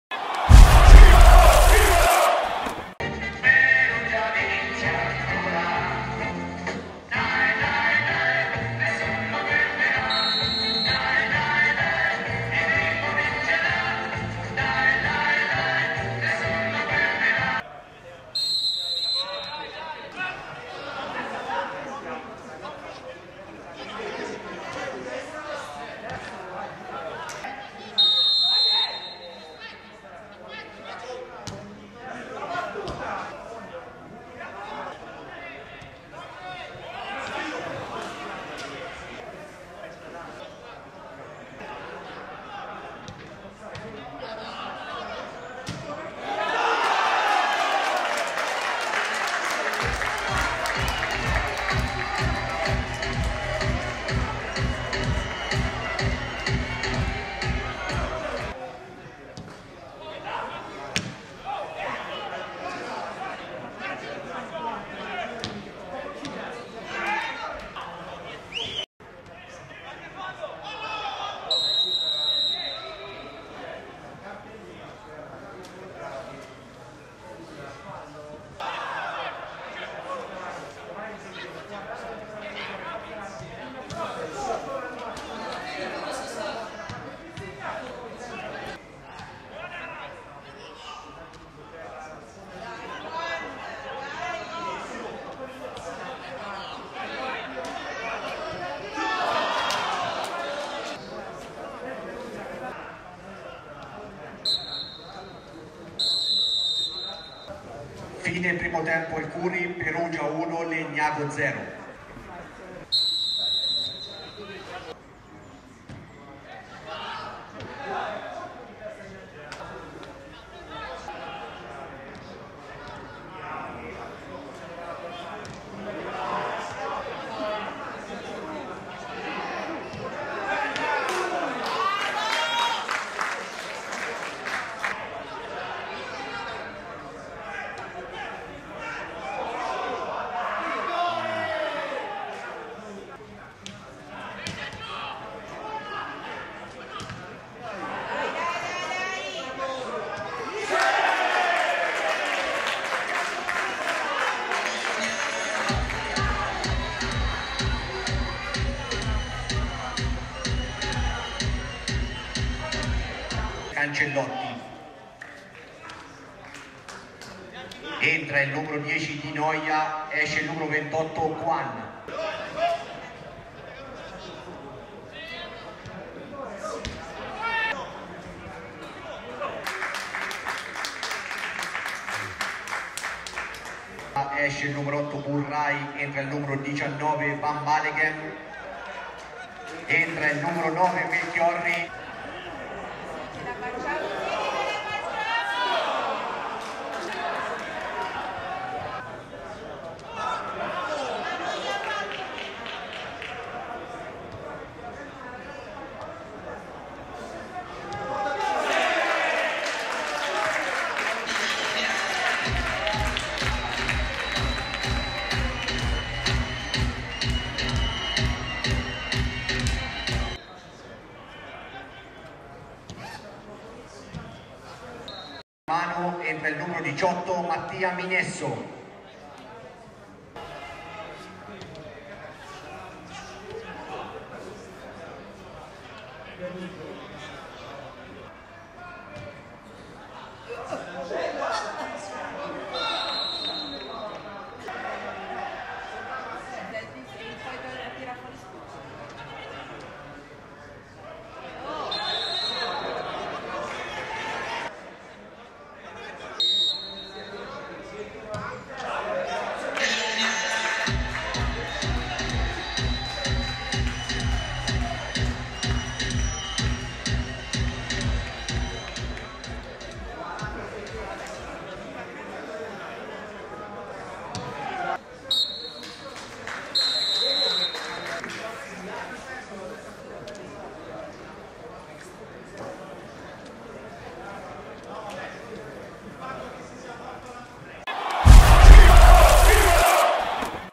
Perugia-Legnago 4-0, highlights
La doppietta di Elia, la rete di Bianchimano e il sigillo di Melchiorri su rigore.